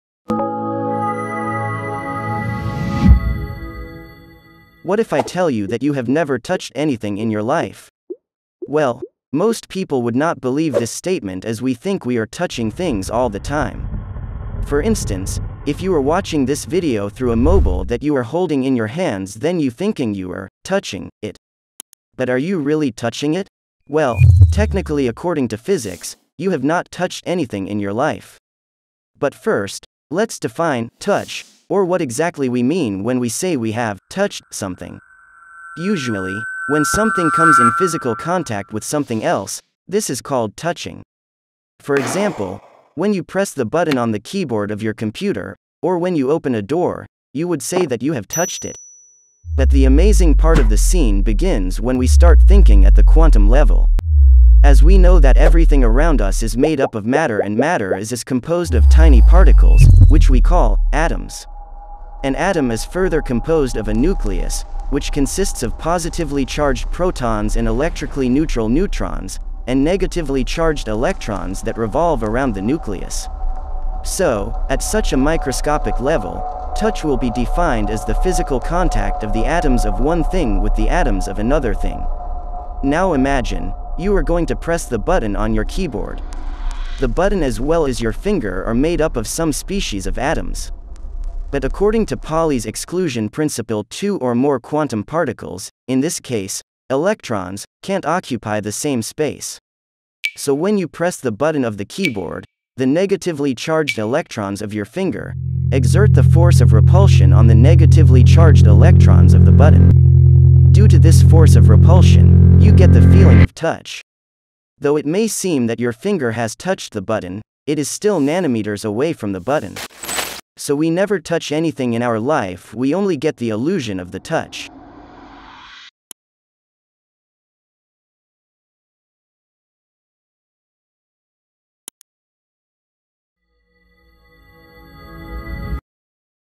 You Can Never Actually Touch Anything
what if i tell you that you Have never touched anything in your life? Well, most people will respond would not beleive tis statement as we think we are touching things all the time. For instance, if you are watching this video through a mobile that you are holding in your hands and you think that you are “touching” it. But are you?

Well, technically According to physics,you have not touched anything in your life.
But first, let’s define “touch” or what exactly we mean when we say we have “touched” something. Usually, when something comes in physical contact with something else, this is called touching. For example, when you press the button on the keyboard of your computer, or when you open a door, you would say that you have touched it. But the amazing part of the scene begins when we start thinking at the quantum level.
as We know that everything around us is made up of matter and matter is composed of tiny particles, which we call “Atoms”. an atom is further composed of a nucleus, which consists of positively charged Protons and electrically neutral neutrons, and negatively charged electrons that revolve around the nucleus. So, at such a microscopic level, touch will be defined as the physical contact of the atoms of one thing with the atoms of another thing.
Now imagine, you are going to press the button on your keyboard.
The button as well as your finger are made up of some species of atoms.
 but according to Pauli’s Exclusion principle two or more quantum particles, in this case, electrons, can’t occupy the same space.
so When you press the button of the keyboard, the negatively charged electrons of your finger, exert the force of repulsion on the negatively charged electrons of the button.Due to this force of repulsion, you get the feeling of touch. Though it may seem that your finger has touched the button, it is still nanometers away from the button.so we never touch anything in our life we only get the illution of the touch.